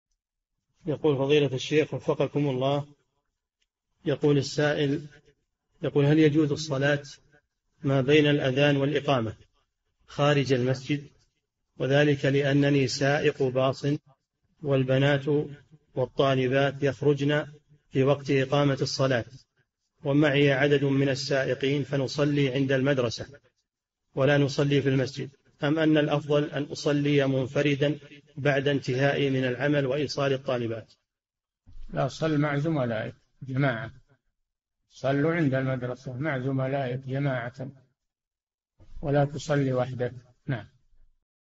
[4084- 4717] أقوم بتوصيل الطالبات ويخرجن عند الإقامة فهل أصلي مع زملائي في المدرسة أم أصلي منفردا ؟
"[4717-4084] هل تجوز الصلاة ما بين الأذان والإقامة خارج المسجد وذلك لأنني سائق باص والبنات والطالبات يخرجن في وقت إقامة الصلاة ومعي عدد من السائقين فنصلي عند المدرسة ولا نصلي في المسجد؟ أم أن الأفضل أن أصلي منفردًا بعد انتهائي من العمل وإيصالي الطالبات؟
الجَوَابُ: لا، صلّ مع زملائك جماعة، صلوا عند المَدْرَسَة مع زملائك جماعة ولا تصلّ وحدك.

للاشتراك في قناة الشيخ صالح الفوزان
فتاوى المجالس في تفسير المفصل

للاستفسار والإقتراحات والدعم
رئيس الفريق
لمشروع كبار العلماء